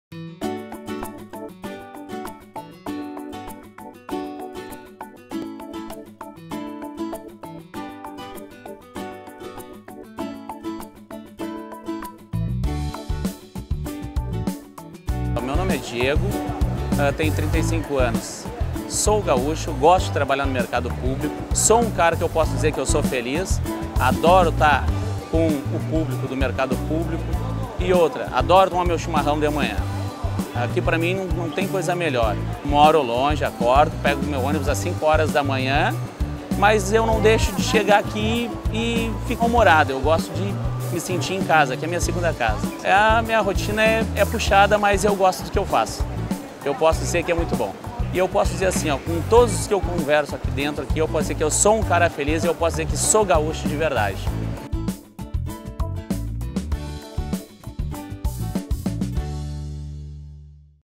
SouGaúchoSouFeliz - Depoimento do balconista Diego Freitas Braga
O balconista do Mercado Público de Porto Alegre, Diego Freitas Braga, fala sobre a alegria de ser gaúcho e trabalhar com o que gosta.